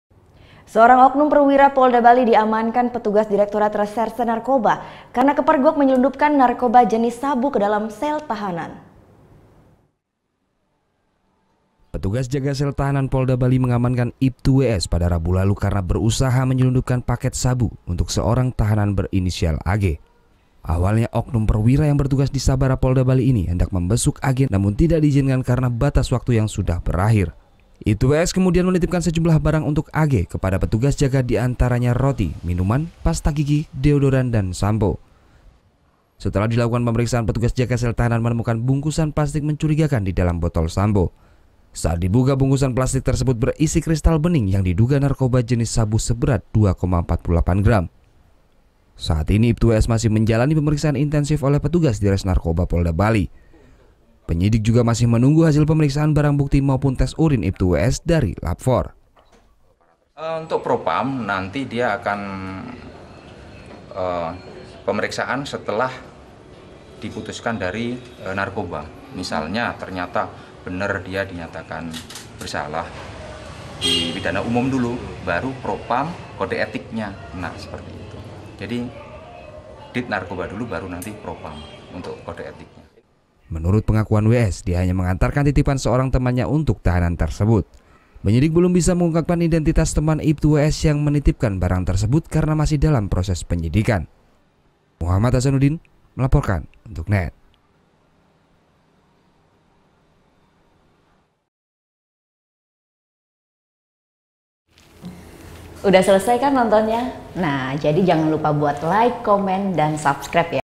NET. BALI - OKNUM PERWIRA POLDA BALI SELUNDUPKAN SABU KE SEL TAHANAN
SEORANG OKNUM PERWIRA POLDA BALI DIAMANKAN PETUGAS DIREKTORAT RESERSE NARKOBA KARENA KEPERGOK MENYELUNDUPKAN NARKOBA JENIS SABU KE DALAM SEL
TAHANAN.